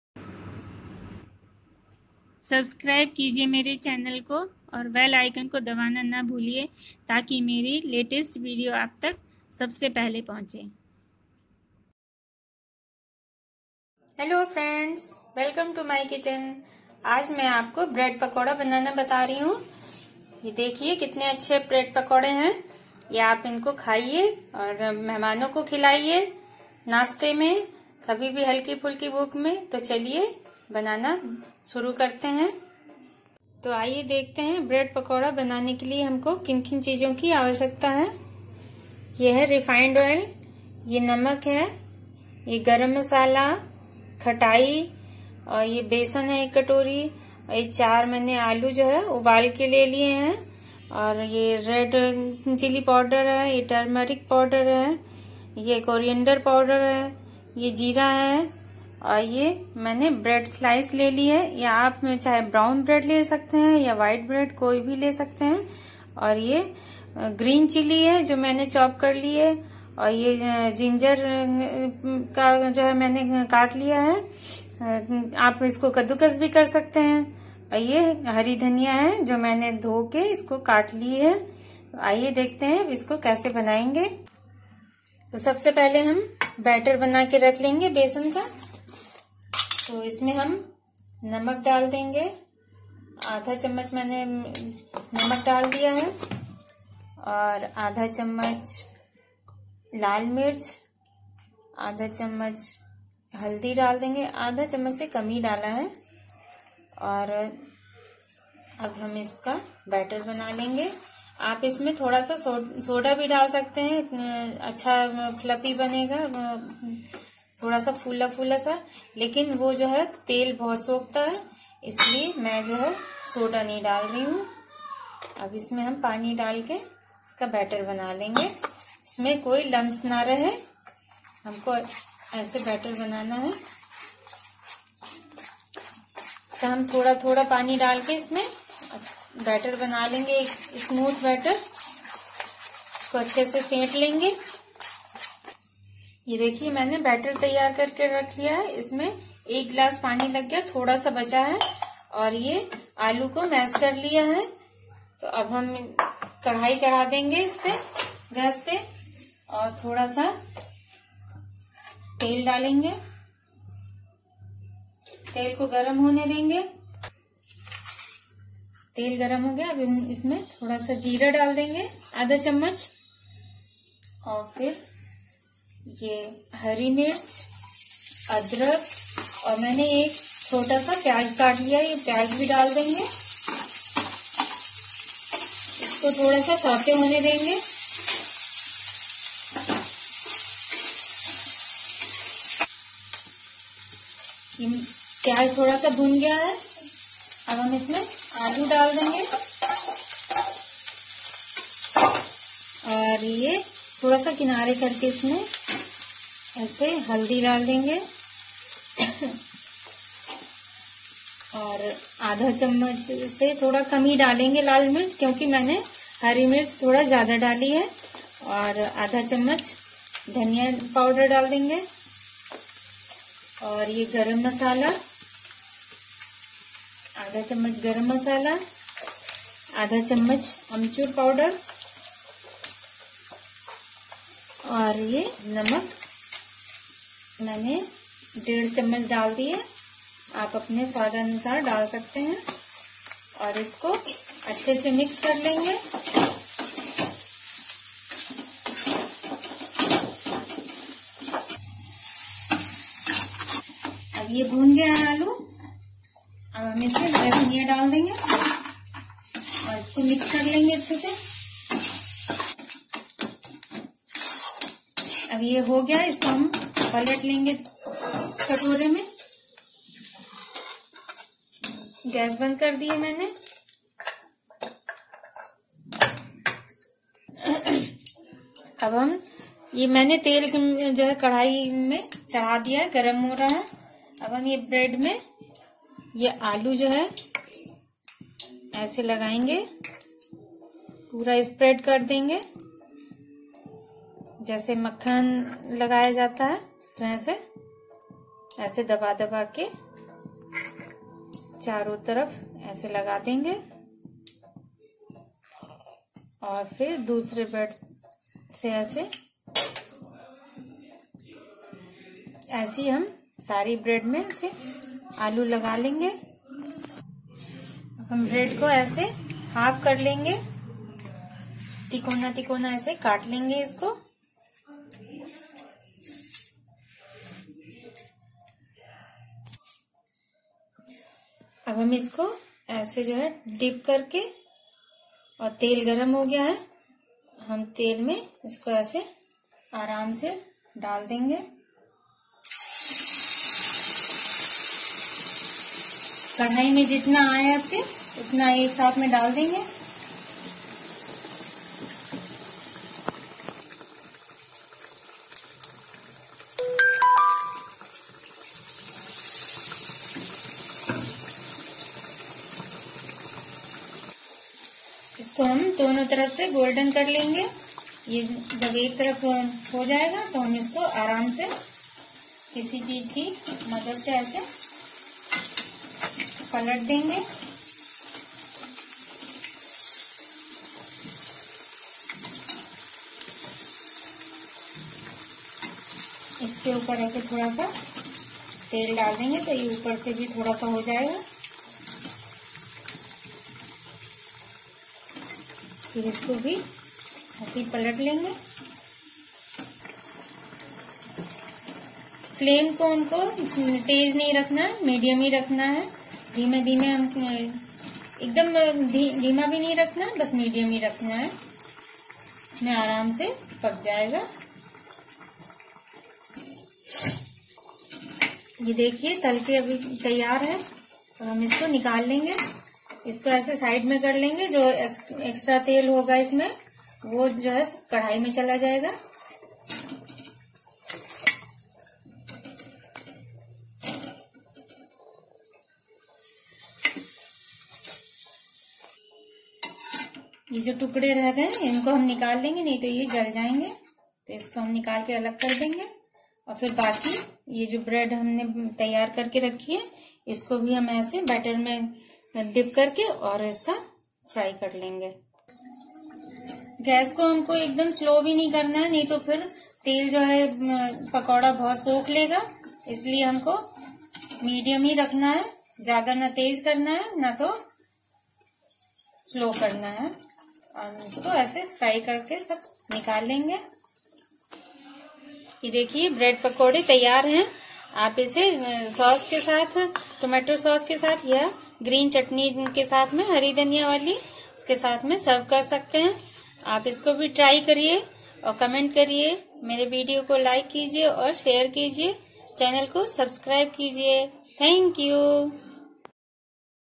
Bread pakora recipe /stuffed bread pakora with potato /आलू भरे ब्रेड पकोड़े कैसे बनाये
Ingredients for 6 pakore

Boiled potato - 4 medium size
Besan - 1 big bowl (150 gm)
Green coriander - 1/2 bowl
Cumin seed - 1/2 teaspoon
Onion - 1
Amchur (dry mango powder) - 1/2 teaspoon
Garam msala - 1/2 teaspoon
Red chilli powder - 1/2 teaspoon
Turmeric power - 1/4 teaspoon
Coriander power - 1/2 teaspoon
Salt to taste
Refined oil for fry

Share family and friends
Comments please 🙏